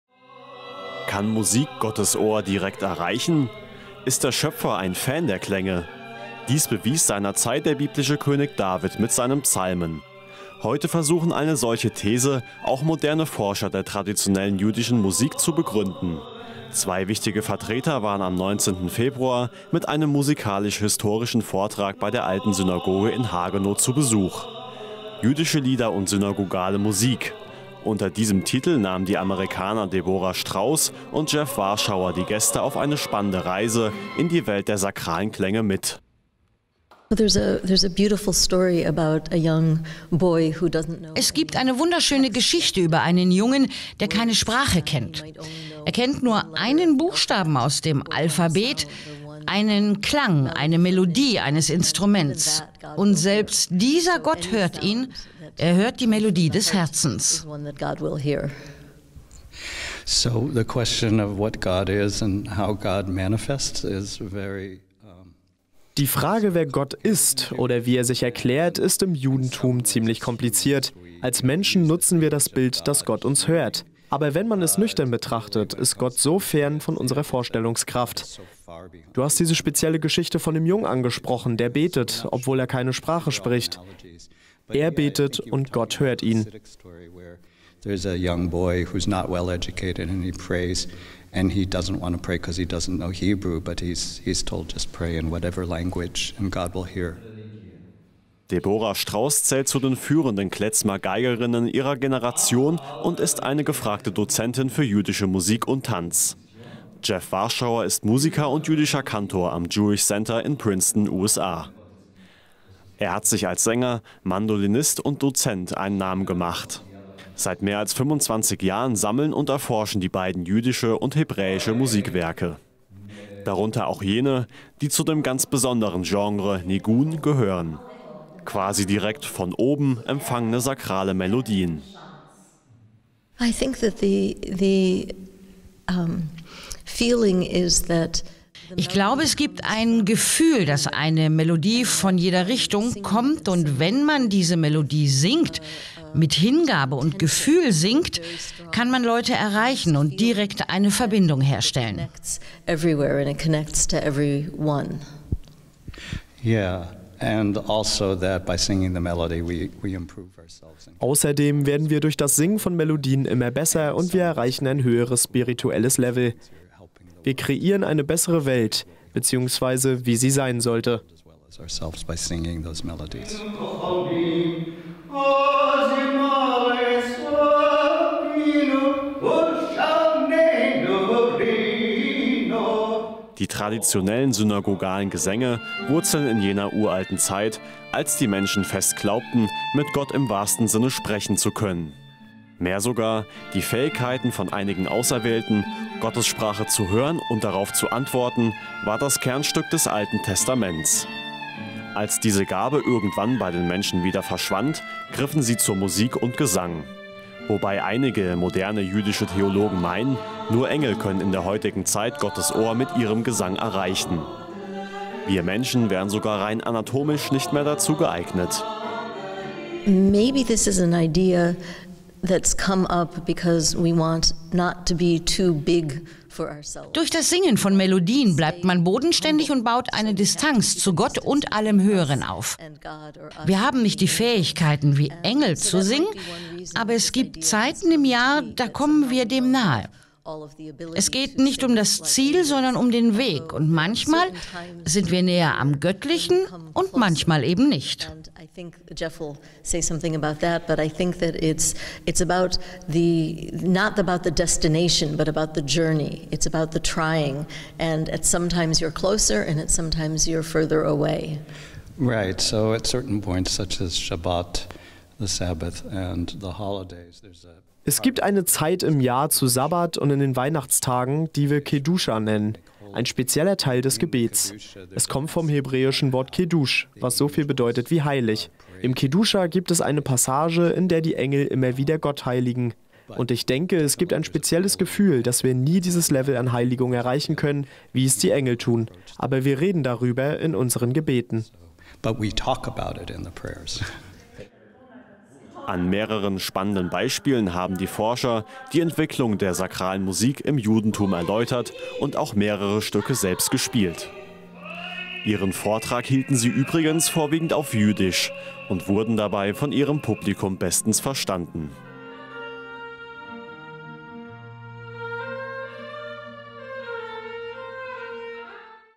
Jüdische Lieder und synagogale Musik
Kann Musik das Gottes Ohr direkt erreichen? Ist der Schöpfer ein Fan der Klänge? Dies bewies seinerzeit der biblische König David mit seinen Psalmen. Heute versuchen eine solche These auch moderne Forscher der traditionellen jüdischen Musik begründen. Zwei wichtige Vertreter waren am 19. Februar mit einem musikalisch-historischen Vertrag bei der Alten Synagoge in Hagenow zu Besuch.